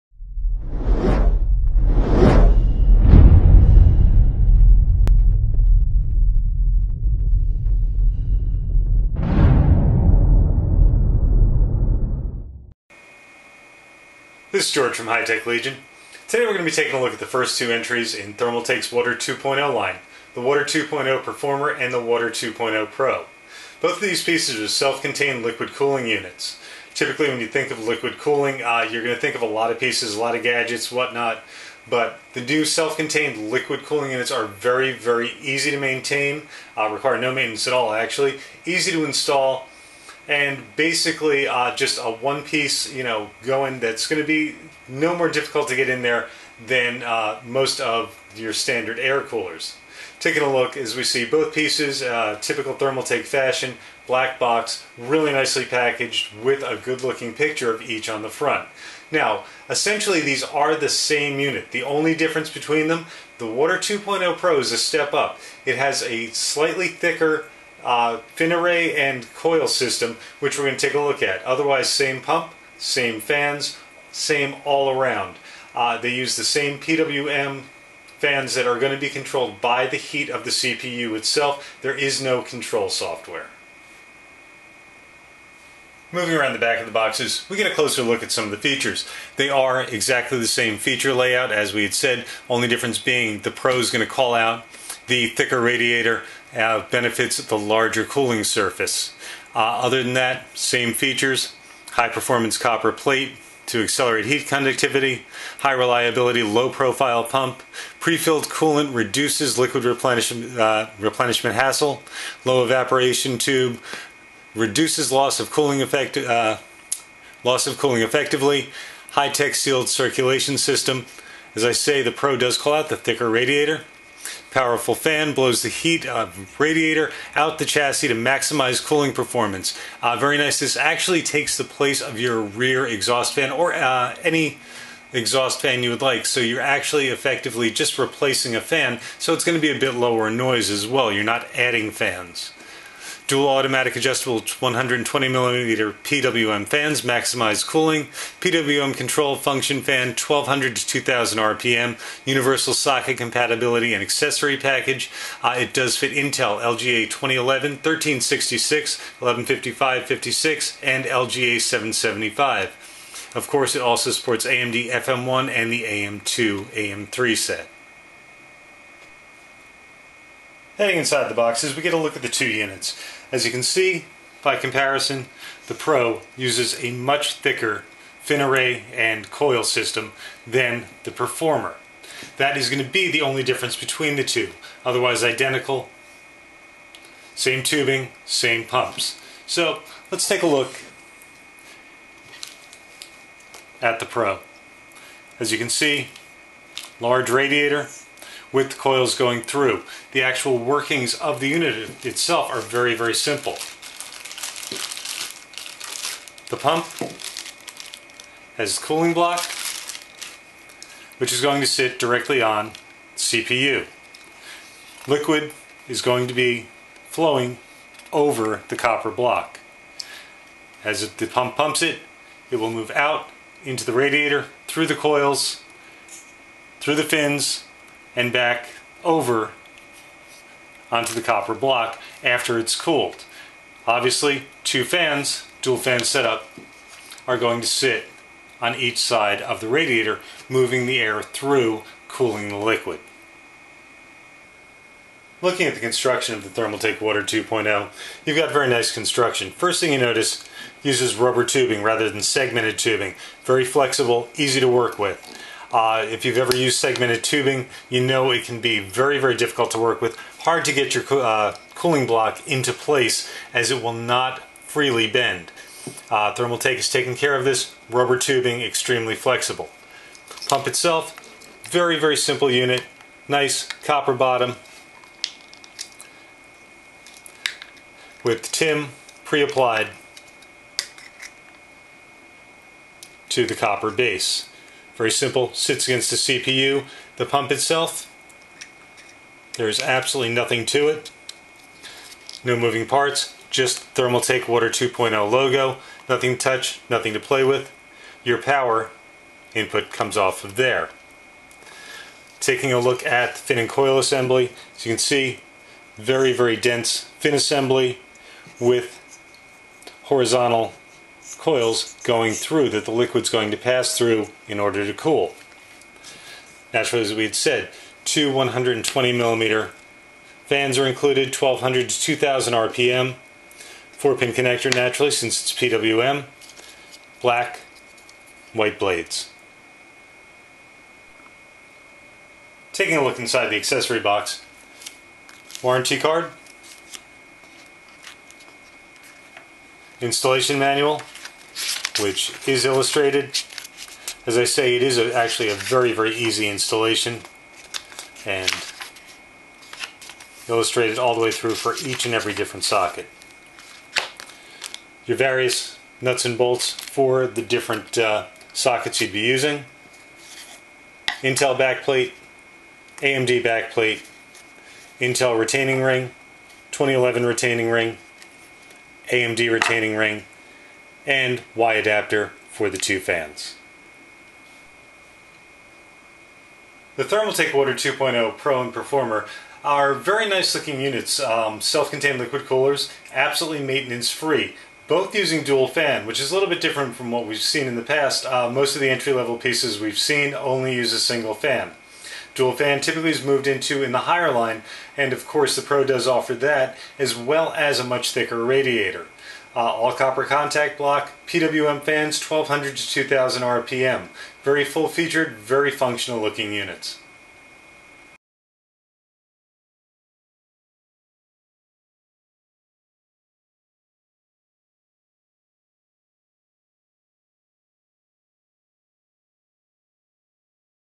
Thermaltake Water2.0 Pro and Performer CPU Coolers Overview
The latest entry in the Self-Contained Liquid Cooling is the Thermaltake Water2.0 series. The initial offerings in the Thermaltake Water2.0 lineup are the base model Performer and the larger upgraded Pro models. Both the Water2.0 Performer and Pro share almost identical features and components, with the Pro upgrading the radiator (fin and coil array) to a thickness doubling the performer for greater heat dissipation for use in more heat intensive applications.